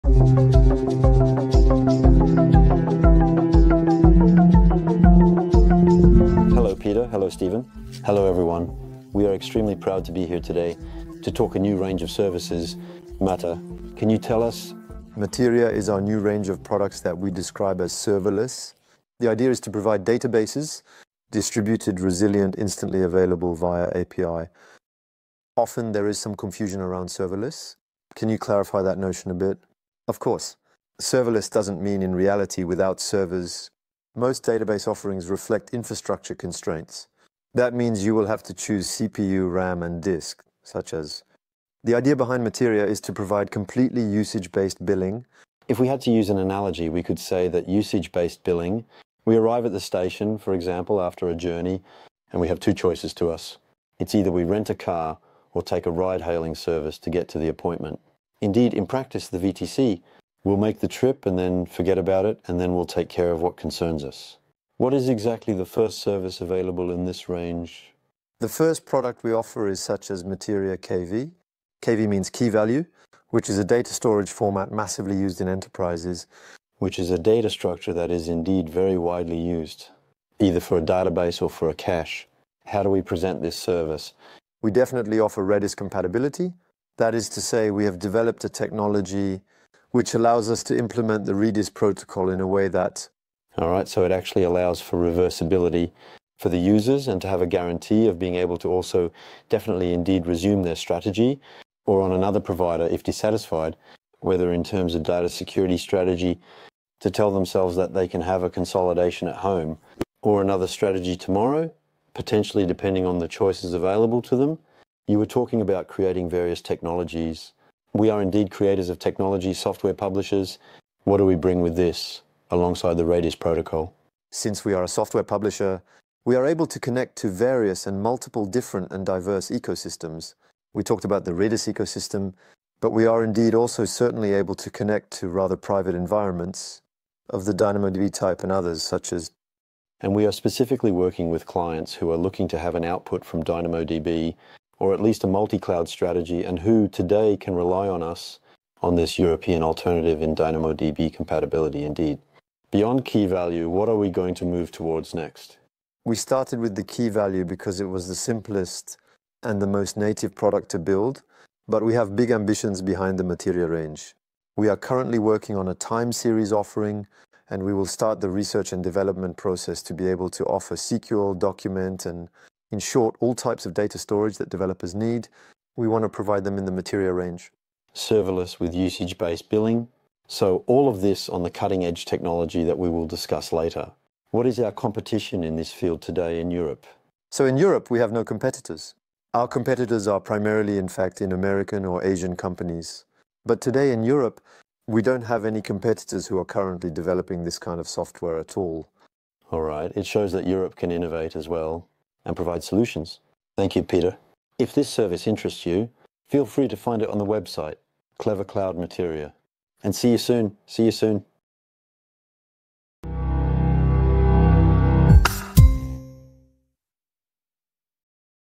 Materia — Serverless Databases
Materia is a range of serverless and managed data storage solutions. Materia grows with your needs, at will.

Materia makes life easier for developers by saving them time. Gone are the days of complex tools and rigid infrastructures, Materia has been designed by and for developers.

Take advantage of an intuitive visual interface to design your data models, while benefiting from a robust, scalable infrastructure.